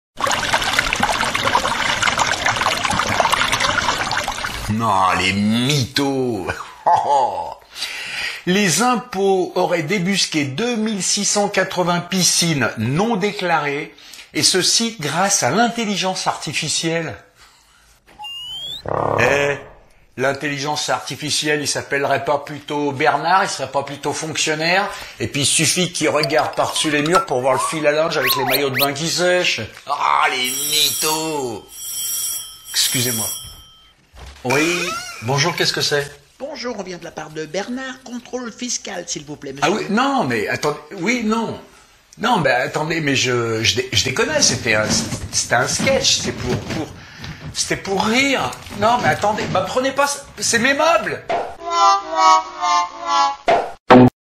Les piscines individuelles et l'intelligence artificielle
Tous ceux qui ont une piscine non déclarée passeront-il a travers les mailles du filet de l'intellgence artificielle .

Merci d'avance  ;-)